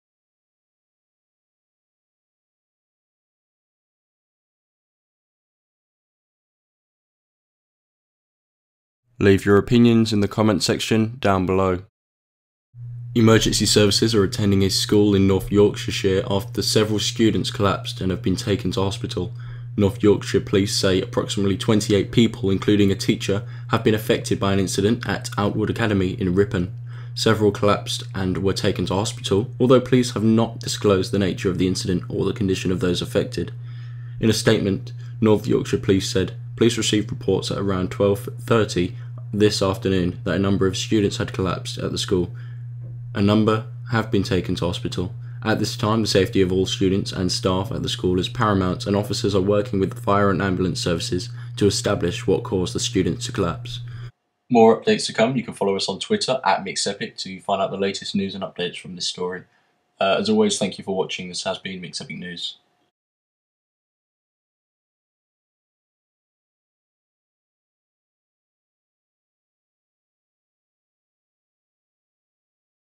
Students Collapse At Yorkshire School
Emergency services are attending a school in North Yorkshire after several students collapsed and have been taken to hospital.

North Yorkshire Police say approximately 28 people including a teacher have been affected by an incident at Outwood Academy in Ripon.

Several collapsed and were taken to hospital although Police have not disclosed the nature of the incident or the condition of those affected.

In a statement, North Yorkshire Police said: "Police received reports at around 12.30 this afternoon that a number of students had collapsed at the school. A number have been taken to hospital.

"At this time, the safety of all the students and staff at the school is paramount and officers are working with the fire and ambulance services to establish what has caused the students to collapse."